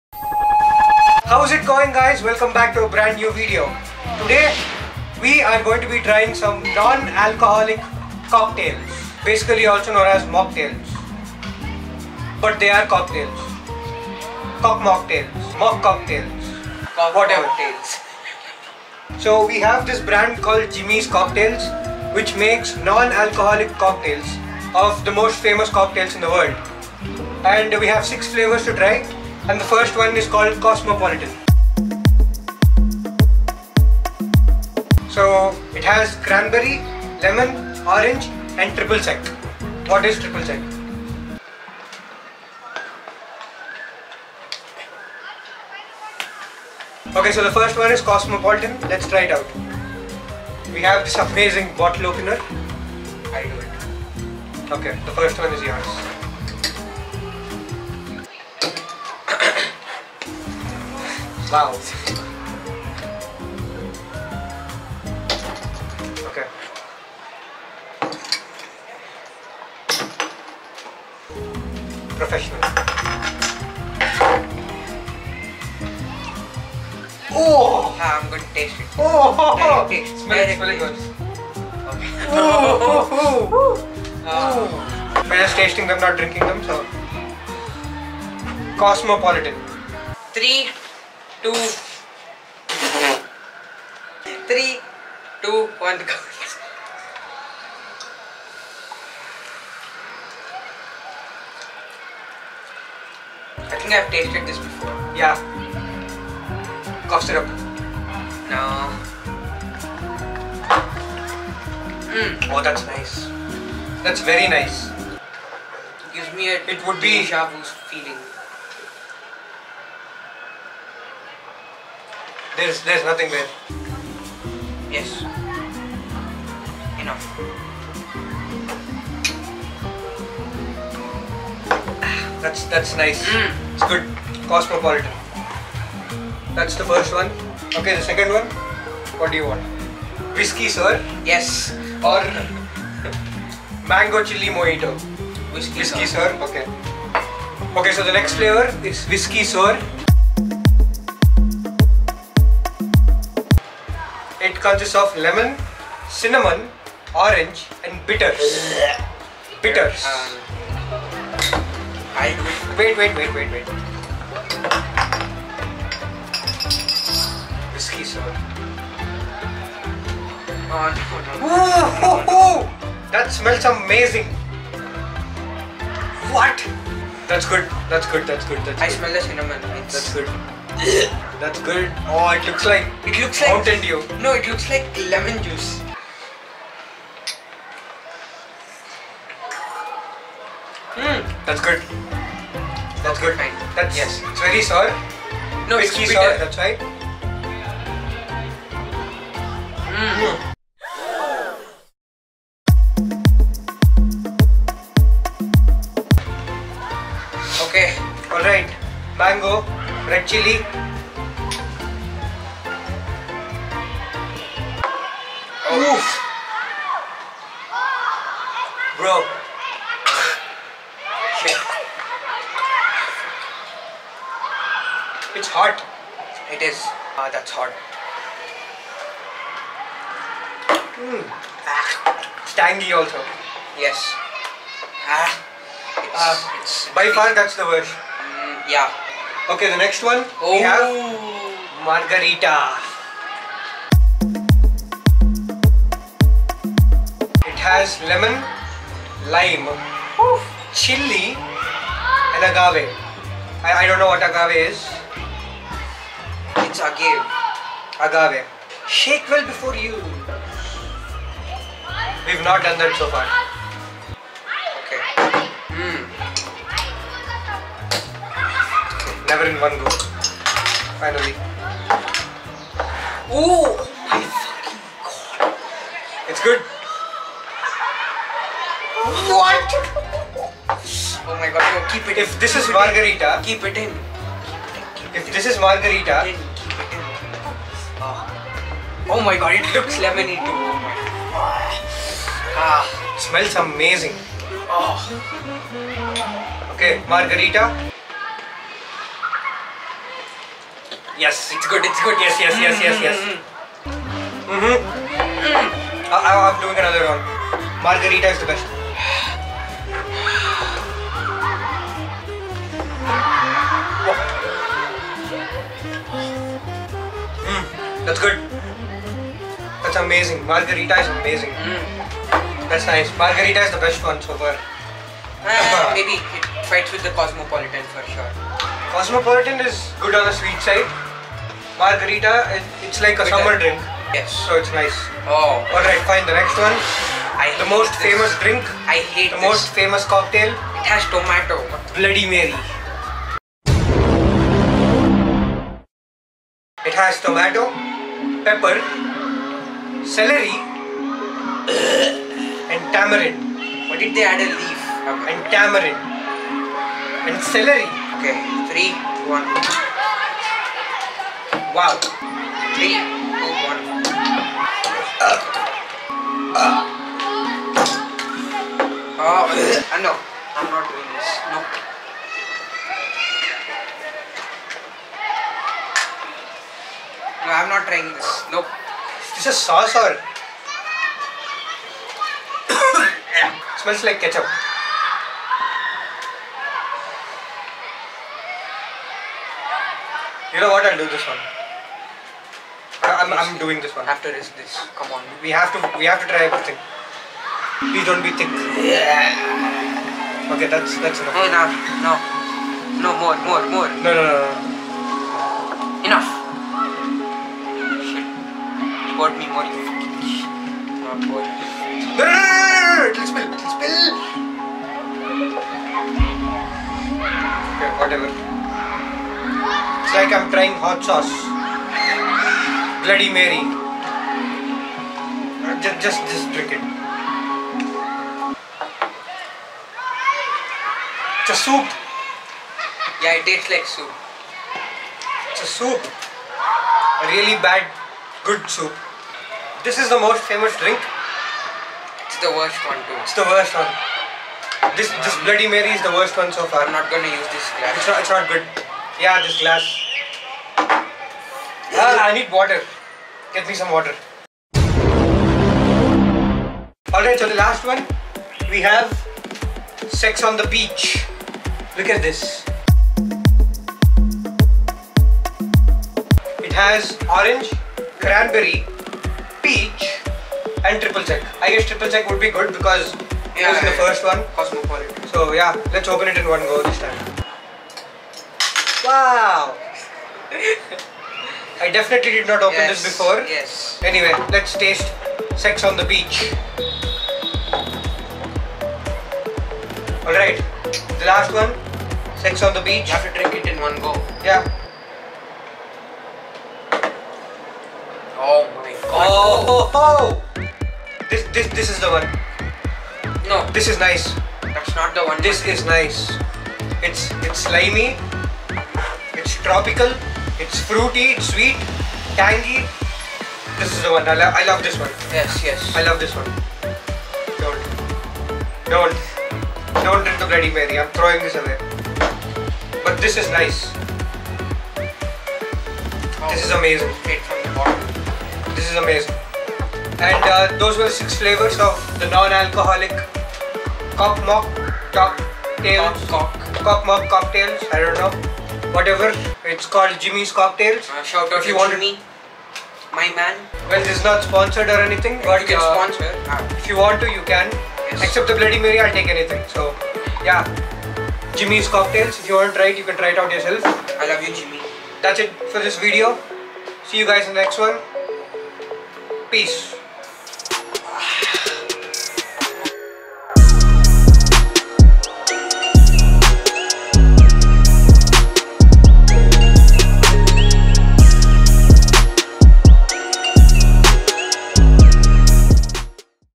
Trying Famous Cocktails For The First Time
Margarita, Bloody Mary, Sex On The Beach, and Whiskey Sour. These are some of the most famous Cocktails in the world.
In this video, I tried famous cocktails and their non-alcoholic versions. Six different flavors, six different experiences.
The best flavor was Sex On The Beach (Of course).
If this video gets 150 likes, I will try alcohol for the first time.